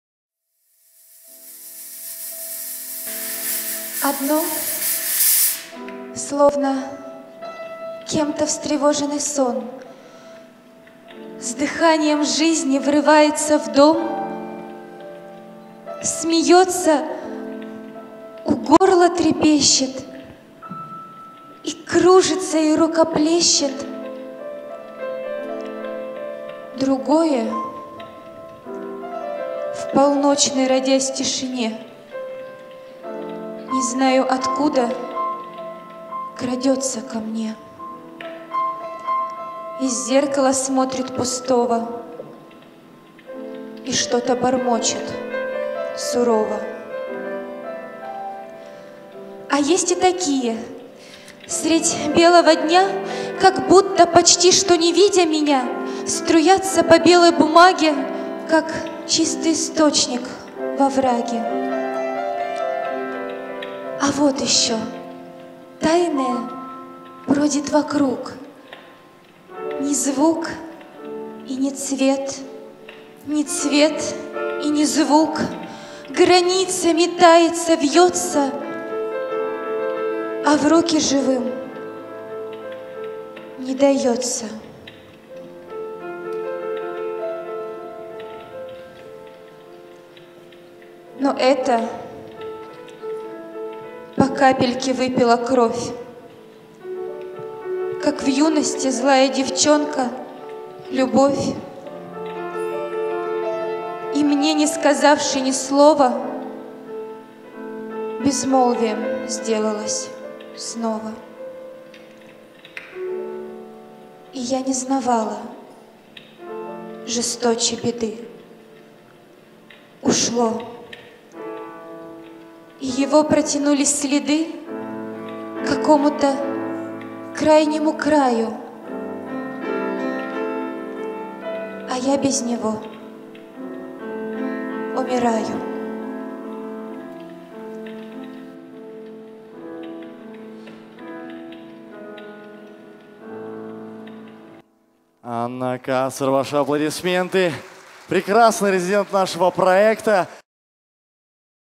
15. Анна Ахматова – Последнее стихотворение (читает Анна Кацер, клавиши: Сергей Достовалов)
// Финальный музыкально-поэтический вечер «Родные РИФМЫ» в рамках проекта РИФМА без границ

РИФМА без границ – первый уникальный проект о поэзии народов России, который проходит в формате культурного шоу: стихи поэтов разных национальностей читают как профессиональные актеры, так и носители языка в сопровождении музыки национальных инструментов, хореографии и других форм искусства. Настоящая магия рифм и образов.

• Дата:  24 ноября 2019 г.
• Место: Сибирский государственный институт искусств (Ленина 22)
• Видео: Дмитрий Тархнишвили, команда Trendvideo
• При поддержке Администрации губернатора Красноярского края, КГАУ «Дом дружбы народов Красноярского края».
• Организаторы: КРОО "Союз армянской молодежи", команда проекта РИФМА без границ